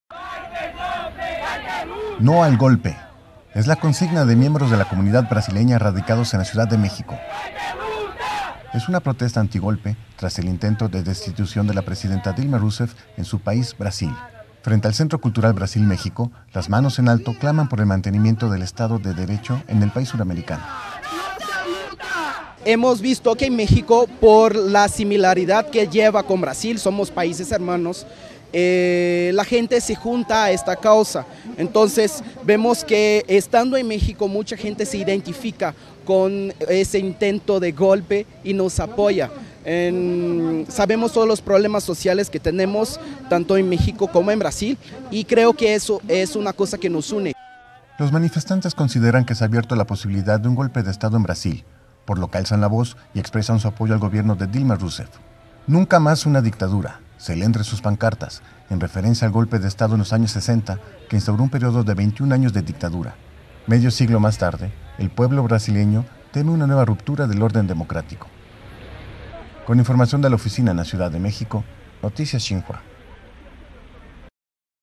“No al golpe”: protesta comunidad brasileña en México
Tras el intento de destitución de la presidenta Dilma Rousseff en Brasil se ha abierto la posibilidad de un golpe de estado en el país suramericano, lo que ha causado manifestaciones en apoyo del gobierno de Rousseff.